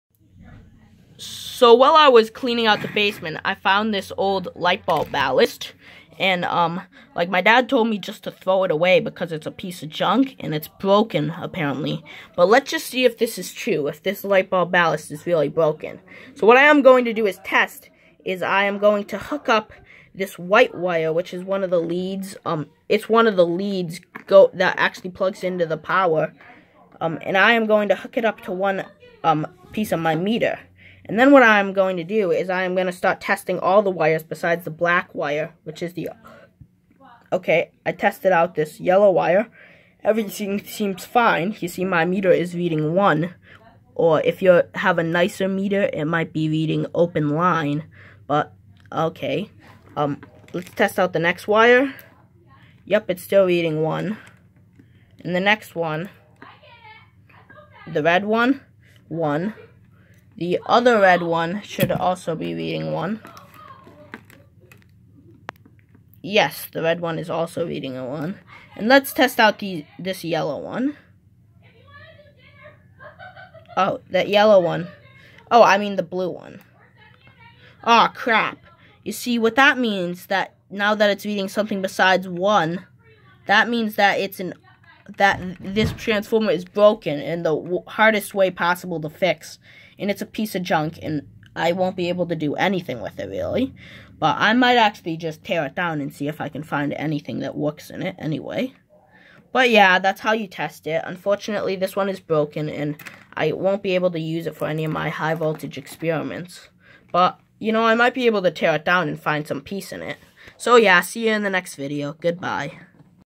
Identifying a broken light ballast
In this video I show you how to tell if your ballast is broken